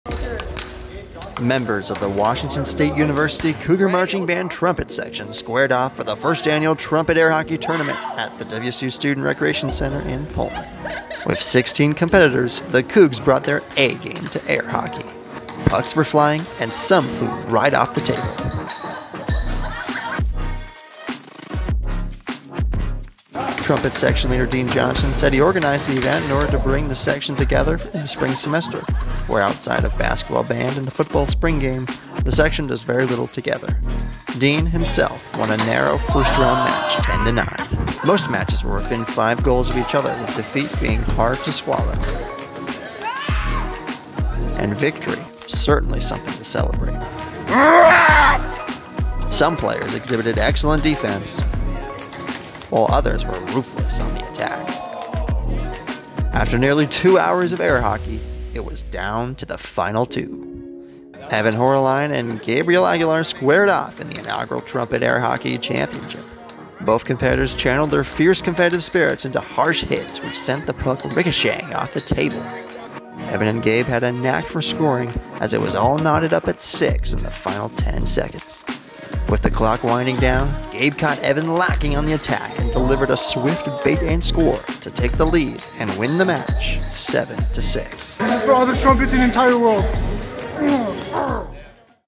WSU Trumpet Air Hockey Championships 2023
In the inaugural meeting of the Cougar Marching Band Trumpet Section's Air Hockey Championships at Washington State University, there is only on winner. Who is it?!?!? The answer is 90 seconds away.